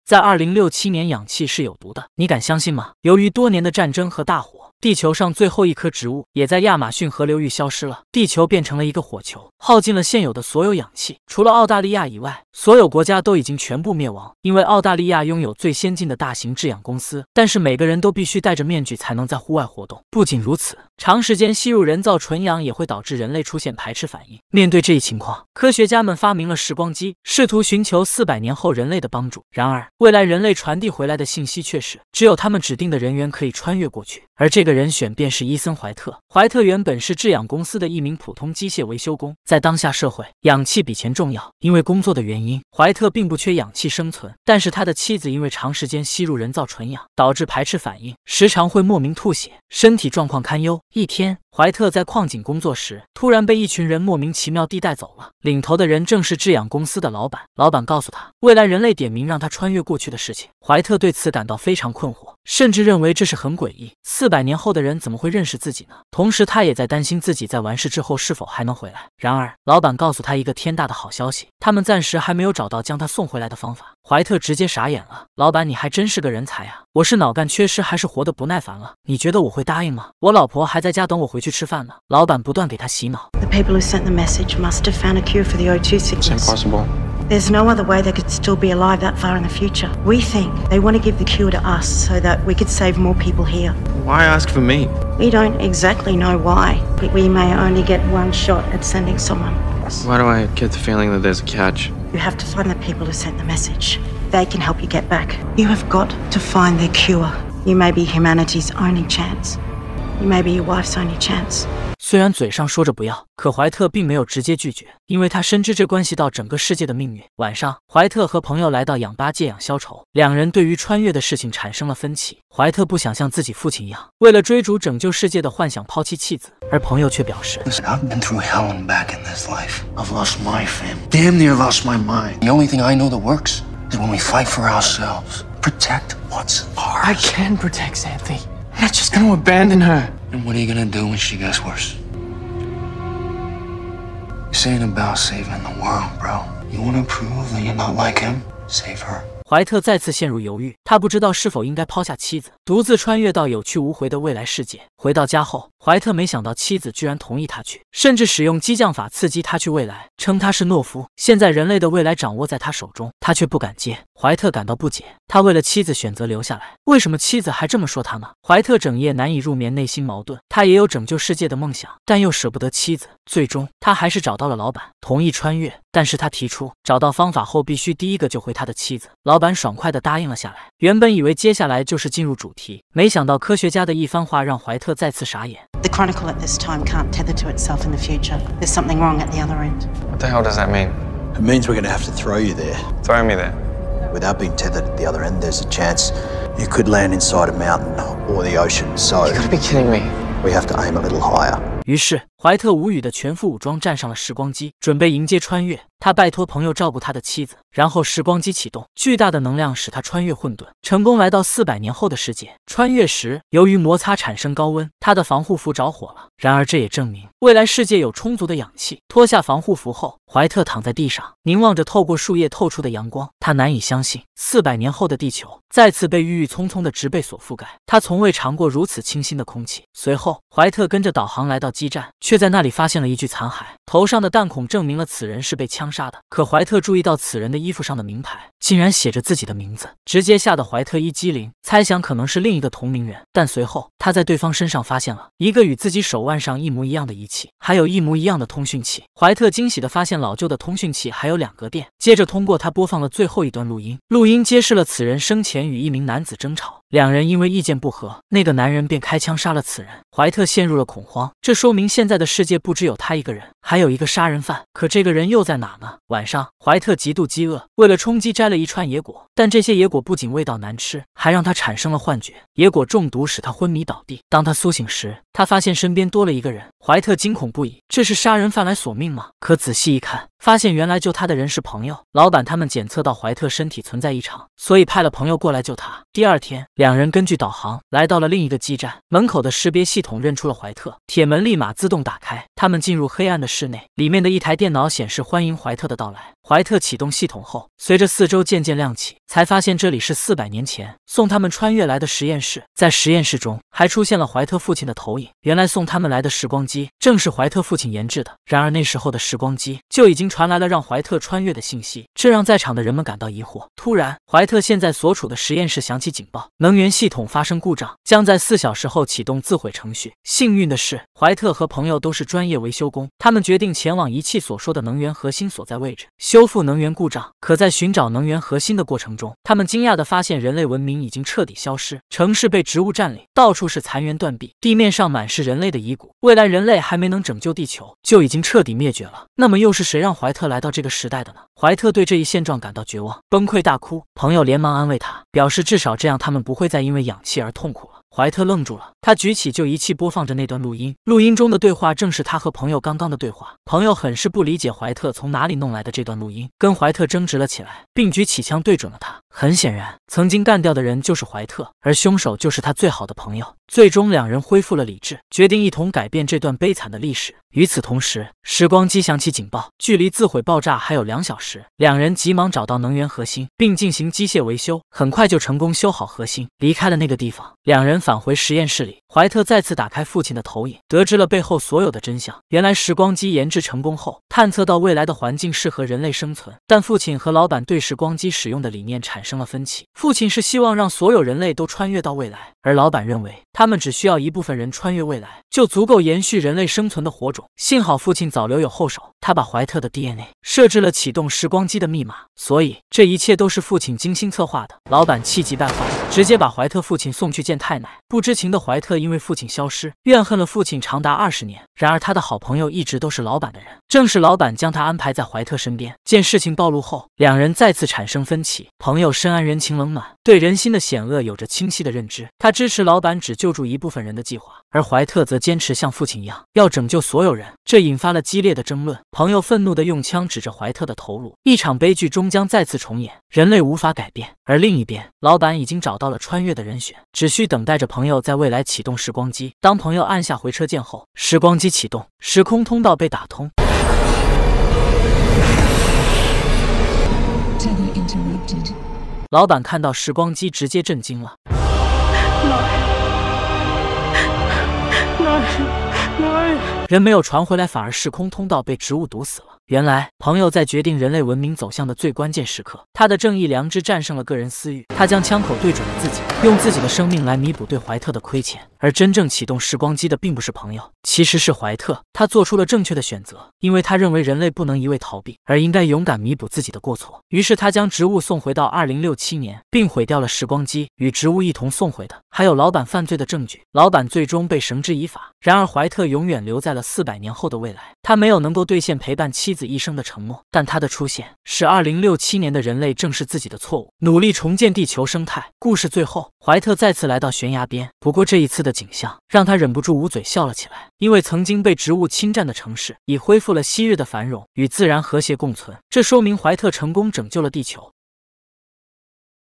【汪哥】在未來世界，氧氣是有毒的，人造氧氣比錢還珍貴！
男人表面清正廉潔，背地卻過皇帝般的生活，誰敢相信他是總統第一候選人